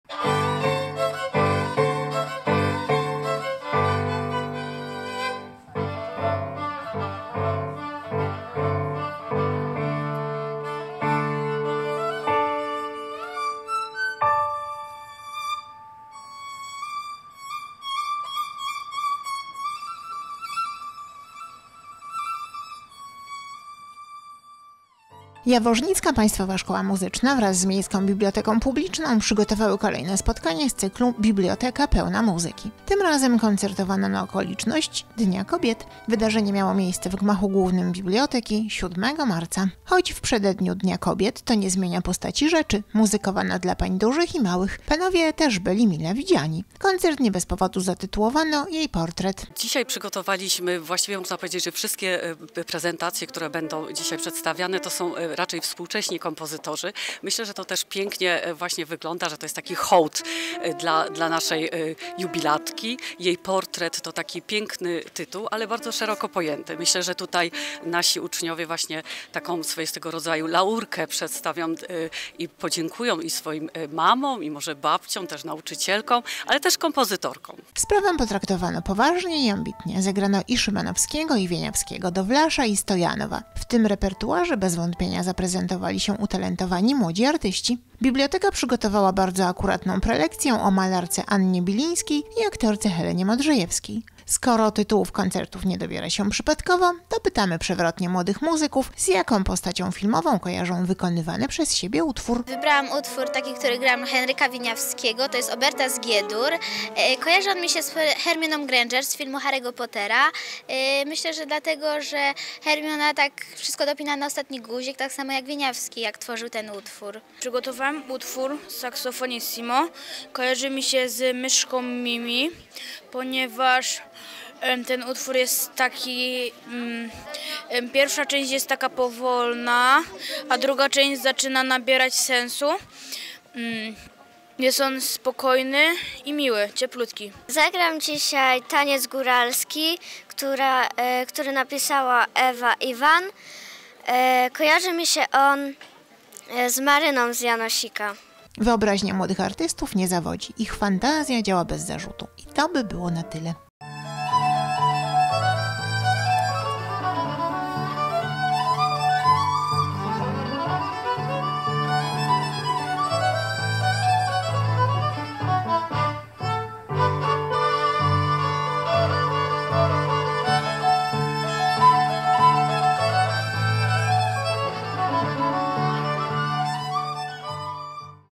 Jaworznicka szkoła muzyczna w koncercie na Dzień Kobiet
Jaworznicka Państwowa Szkoła Muzyczna wraz z Miejską Biblioteką Publiczną przygotowały kolejne spotkanie z cyklu „Biblioteka pełna muzyki”. Tym razem koncertowano na okoliczność Dnia Kobiet. Wydarzenie miało miejsce w gmachu głównym biblioteki 7 marca.

Choć w przededniu Dnia Kobiet, to nie zmienia postaci rzeczy – muzykowano dla pań dużych i małych, panowie też byli mile widziani. Koncert nie bez powodu zatytułowano „Jej portret”.

Sprawę potraktowano poważnie i ambitnie. Zagrano i Szymanowskiego, i Wieniawskiego, Dowlasza i Stojanowa. W tym repertuarze bez wątpienia zaprezentowali się utalentowani młodzi artyści. Biblioteka przygotowała bardzo akuratną prelekcję o malarce Annie Bilińskiej i aktorce Helenie Modrzejewskiej. Skoro tytułów koncertów nie dobiera się przypadkowo, to pytamy przewrotnie młodych muzyków z jaką postacią filmową kojarzą wykonywany przez siebie utwór.

Wyobraźnia młodych artystów nie zawodzi, ich fantazja działa bez zarzutu. I to by było na tyle.